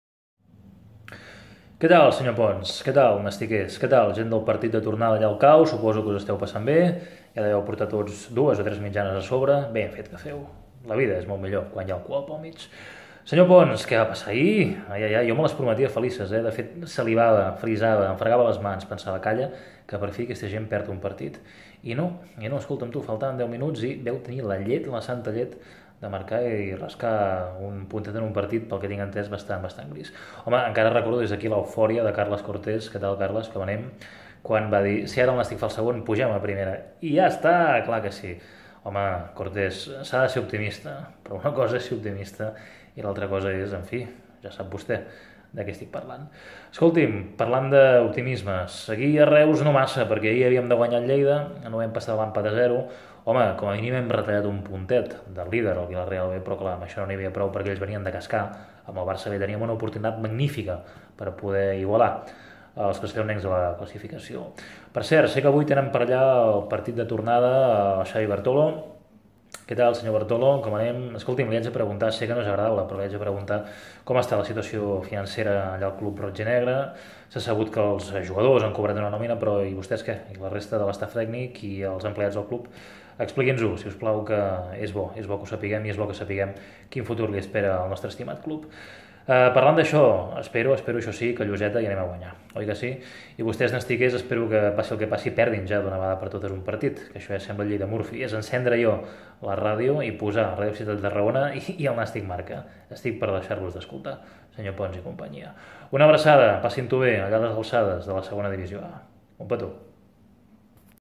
Partit de Tornada núm36. Ganxet Emmascarat
El Ganxet segueix esperant la derrota del Nàstic i aquesta no arriba.